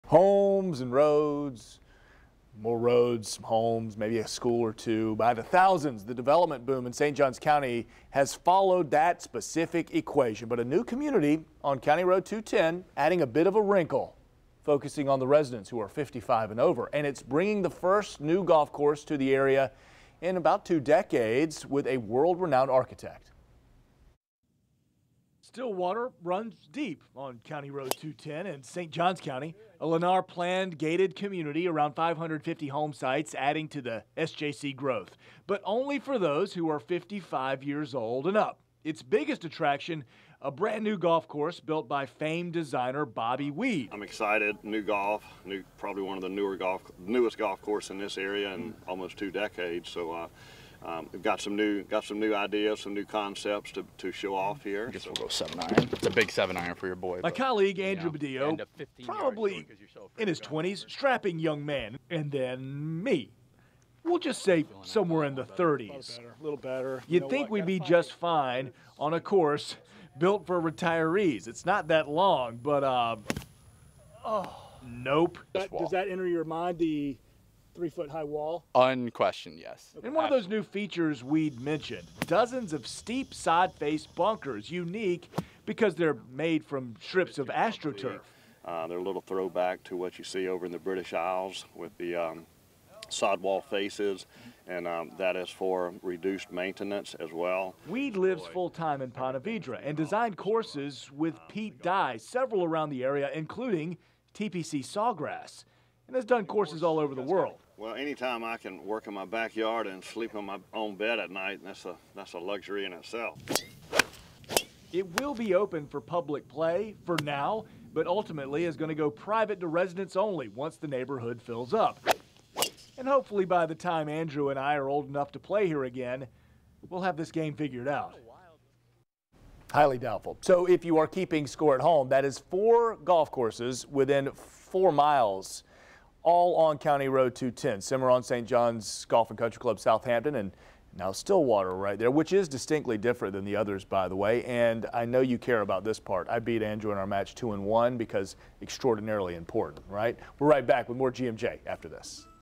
Stillwater brings first new golf course in two decades
The 55 and up community on County Road 210 in St. Johns County will open tee times for public play this summer.